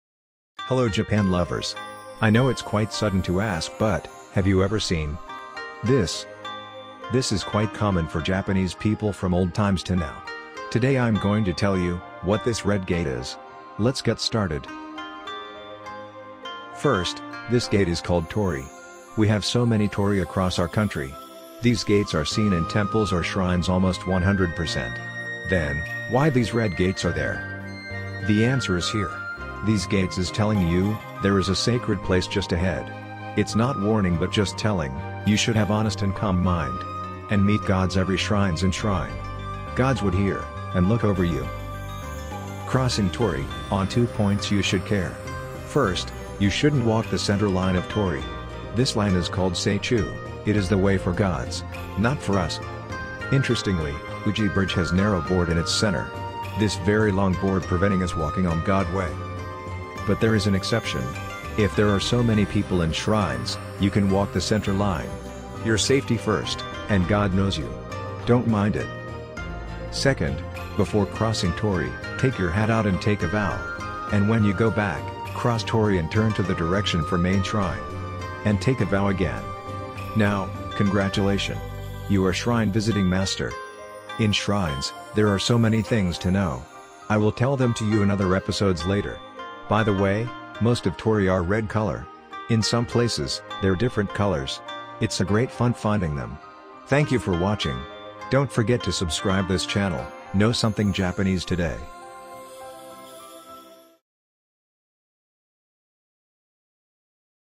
Torii Gate EXPLAINED
You might have seen in movies, animations or visited already.
This time, I will tell you what torii gates are.

Japan is the very country moderns & traditionals are come along.

Chapter list:
00:00 Intro
00:15 Introduction of Torii
00:25 Why Torii gates are in Sacred area?
00:41 Small Tips for Crossing Torii
00:52 God way
01:22 Info for Upcoming Videos
01:30 Unique colored Torii-gates
01:35 Ending